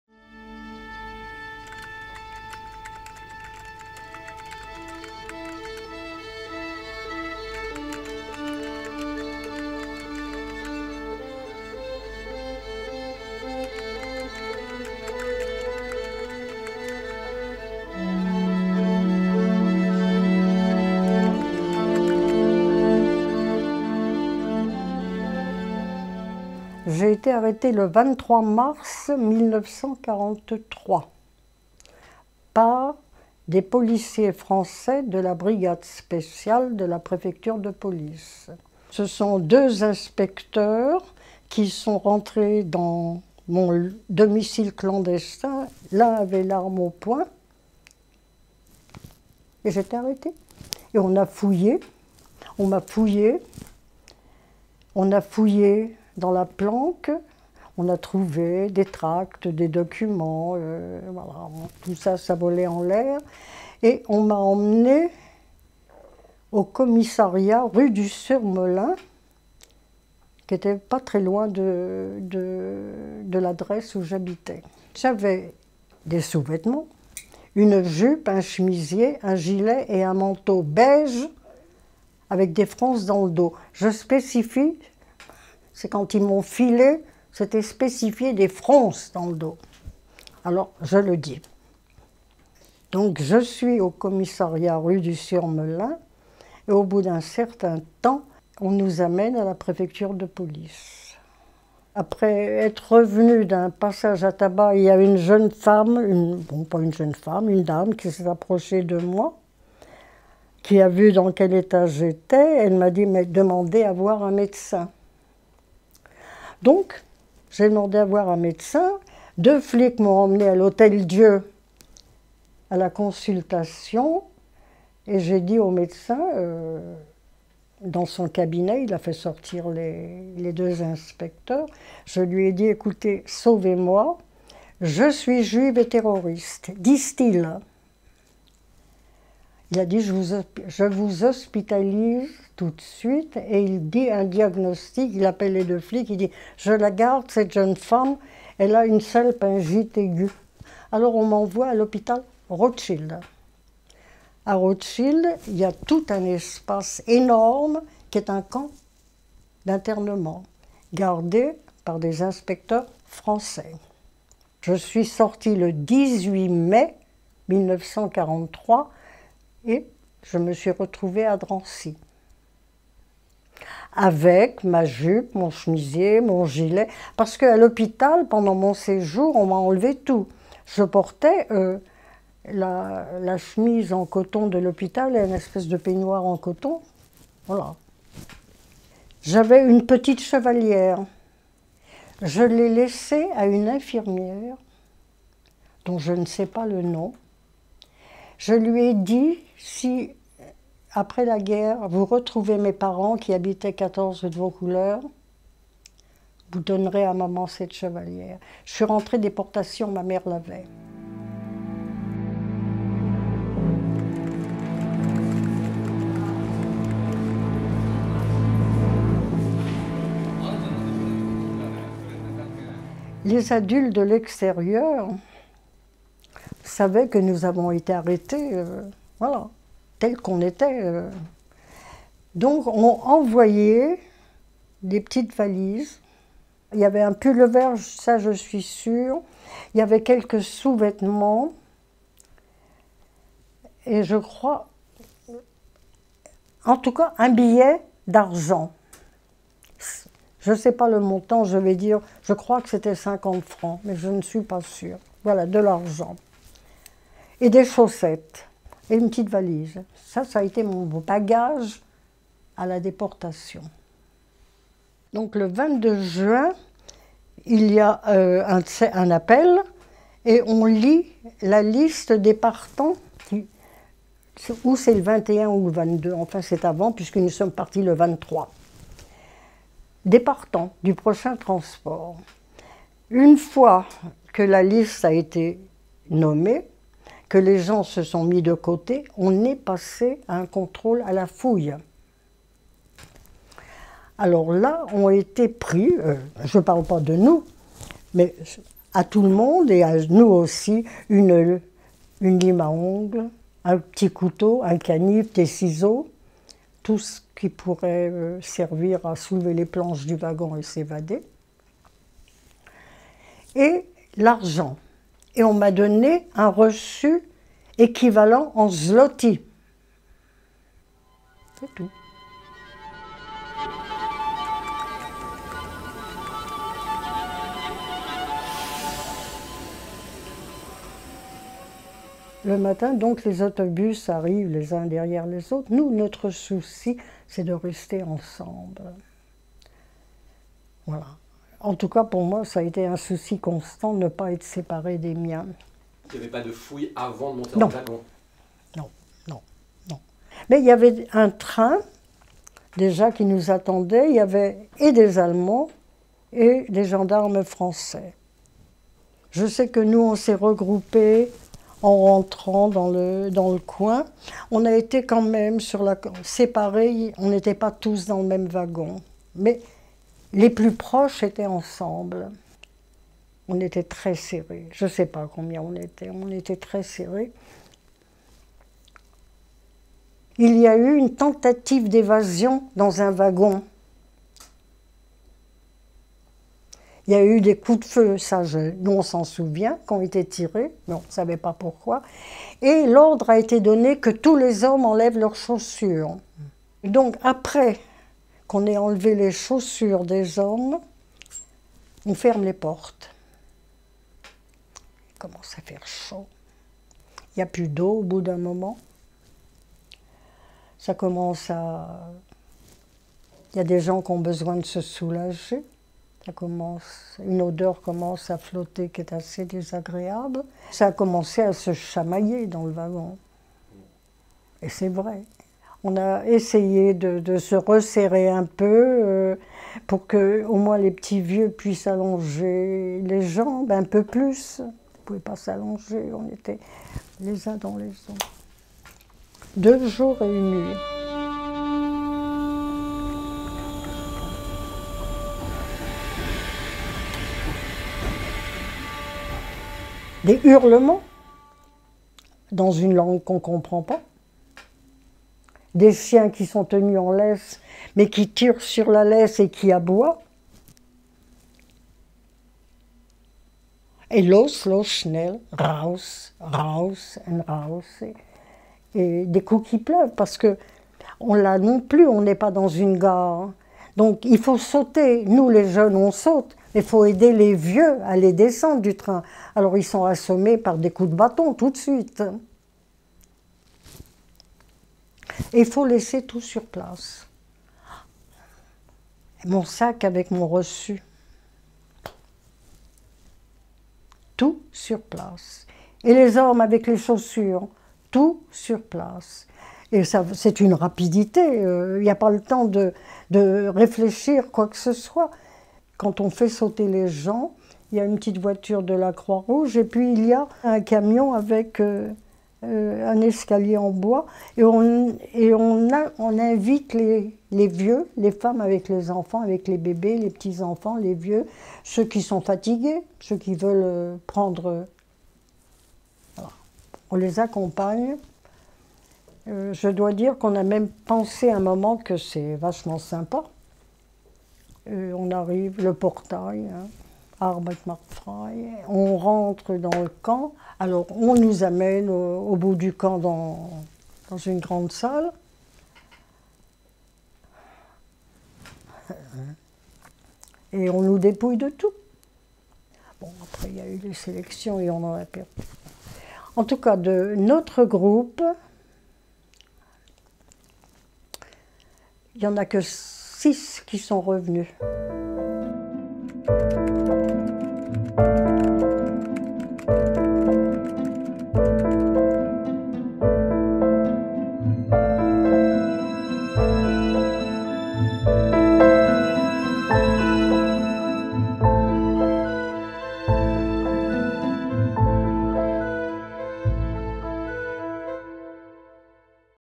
Témoignage de Paulette Sarcey, rescapée de la Shoah
En septembre 1939, Paulette Sarcey avait 15 ans et vivait à Paris. Le 23 mars 1943, elle est arrêtée chez elle.